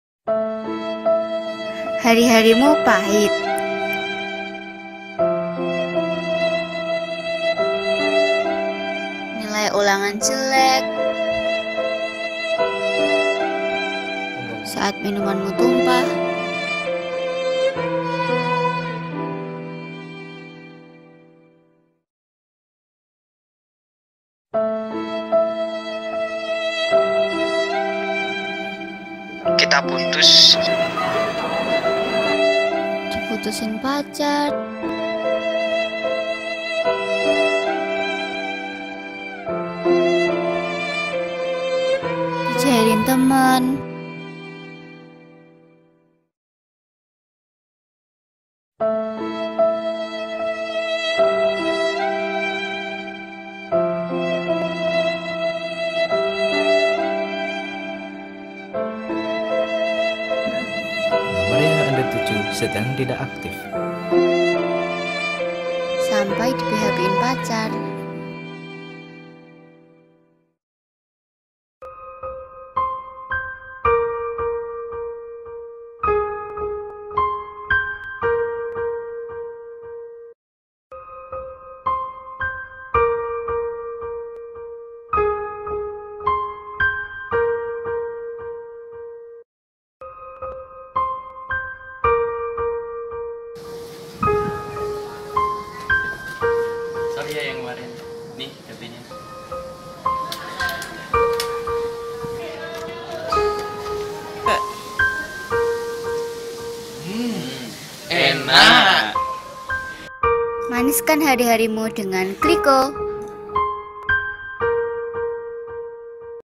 Iklan Makanan Khas Daerah "Tili Aya - GLYCO"
Dibuat oleh kelas 11.IPA.3 SMA N 2 Semarang
Kelompok Dyfan, Rif'ah, Farah, Juven, Lintang, Syifa